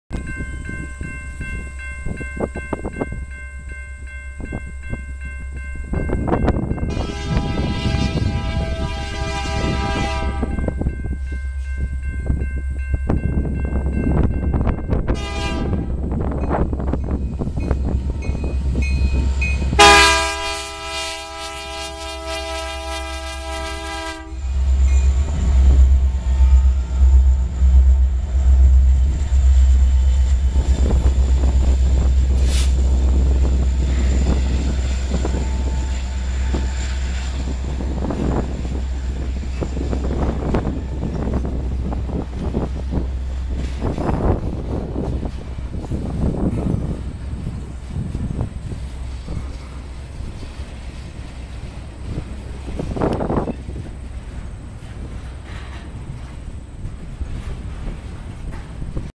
HD KCS south from Fostoria, Ohio on CSX main 014.MOV
KCS stacks, south out of Fostoria, Ohio